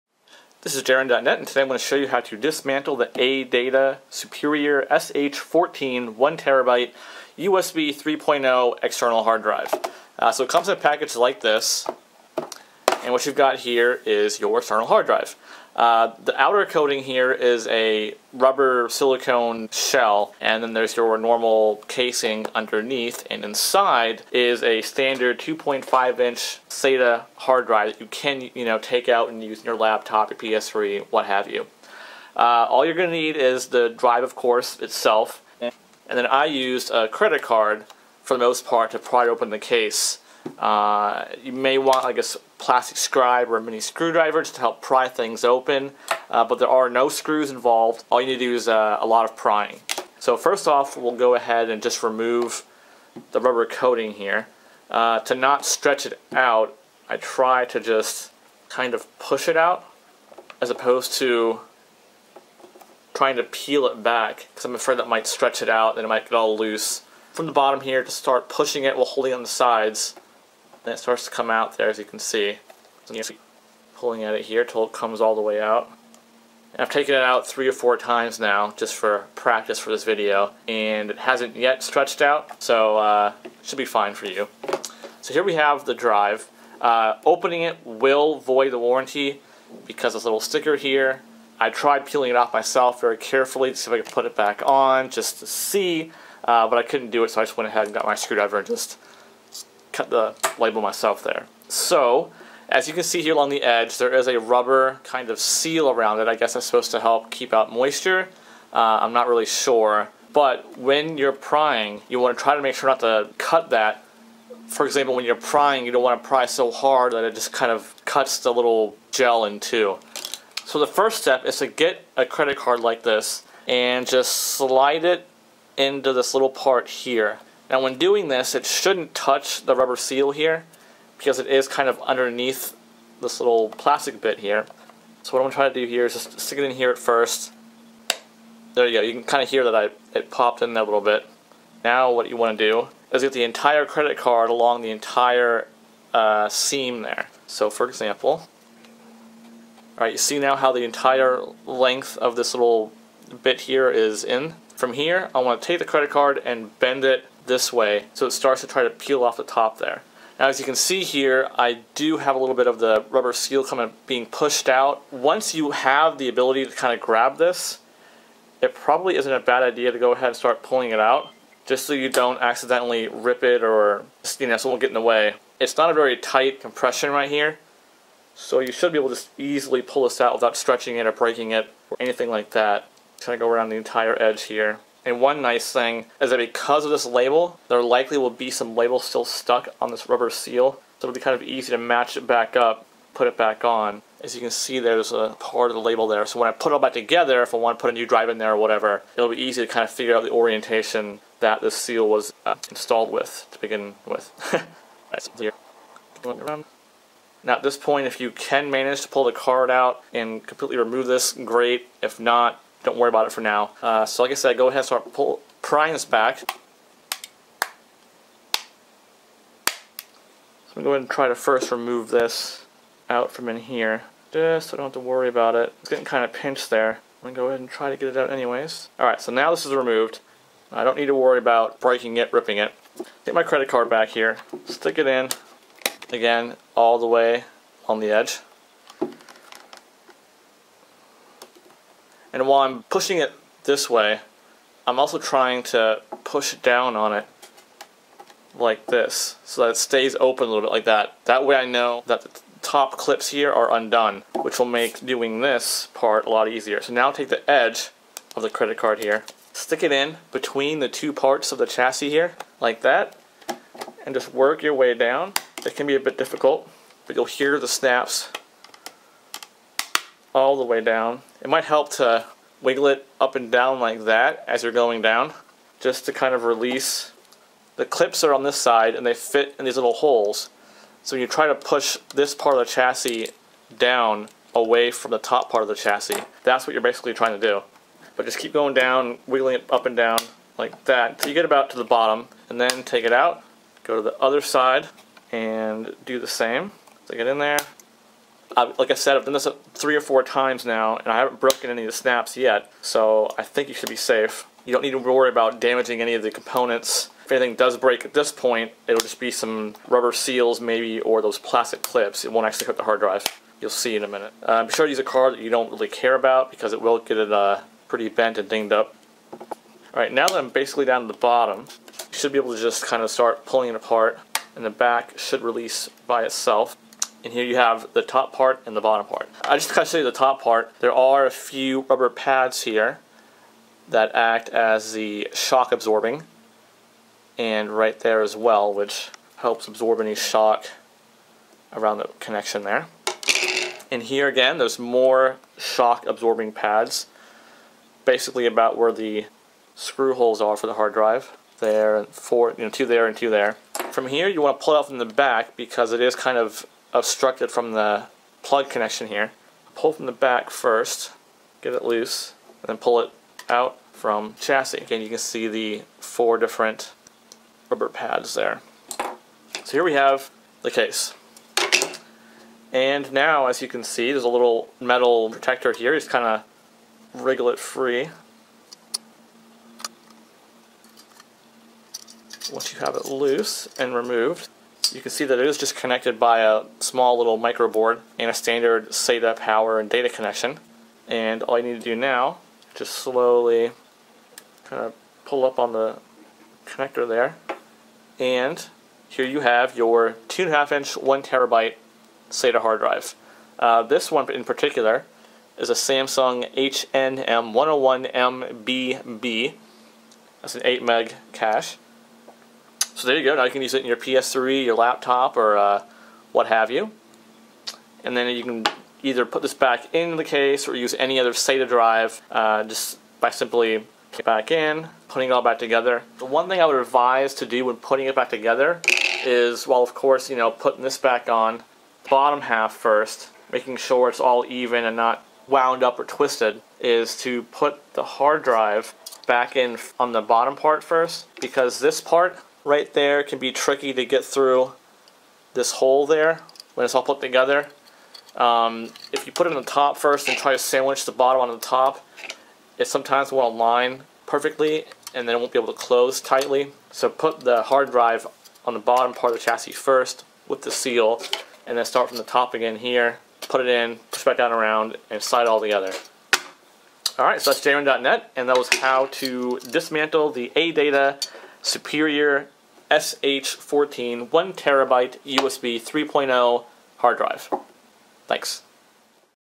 How to dismantle an A-Data SH14 1TB External Hard Drive
jRin.net shows you how to dismantle / take apart the A-Data SH14 1TB USB 3.0 External USB Hard drive